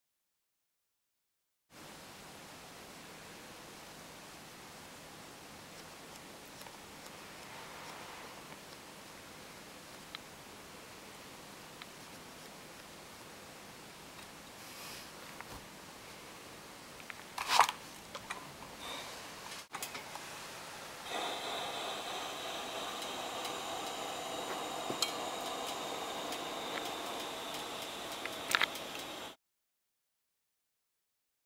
Creating Bias Tape Tubes
This short video demonstrates how to insert fabric strips into the Clover Bias Tape Maker and how to use the Bias Tape Maker with an iron to make bias tape tubes for Santa Sac or other fabric bag drawstrings